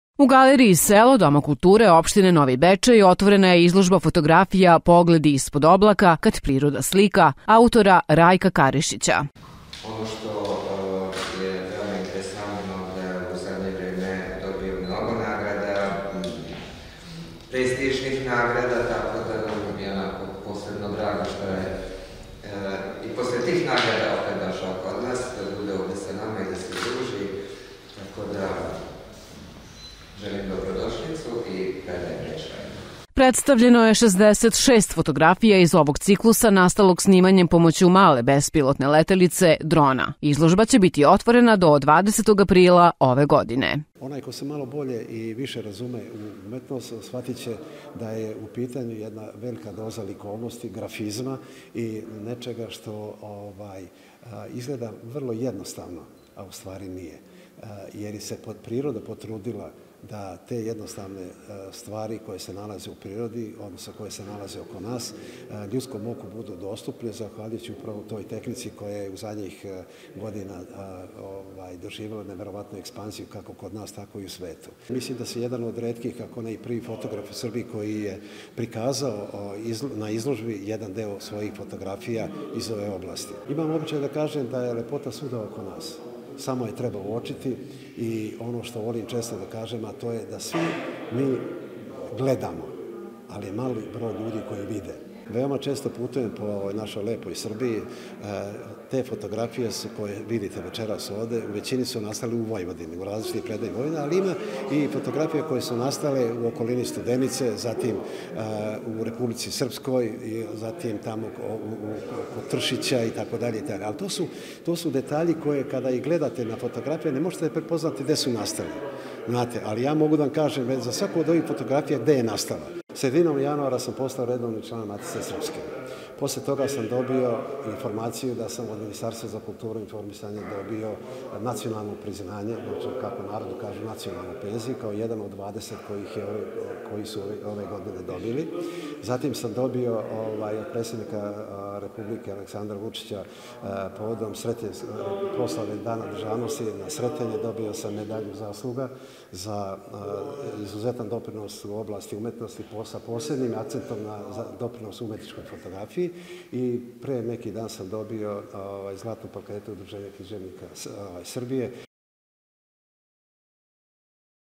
Otvorena izložba "Pogledi ispod oblaka - kad priroda slika", autora Rajka Karišića"
U Galeriji "Selo" Doma kulture opštine Novi Bečej otvorena je izložba fotografija "Pogledi ispod oblaka - kad priroda slika", autora Rajka Karišića.
Predstavljeno je 66 fotografija iz ovog ciklusa nastalog snimanjem pomoću male bespilotne letilice - drona.
Prisutne lјubitelјe fotografije pozdravila je, u ime domaćina, Edit Zerebenji Tenji, dok je o izloženim fotografijama govorio sam autor. Izložba će biti otvorena do 20. aprila ove godine.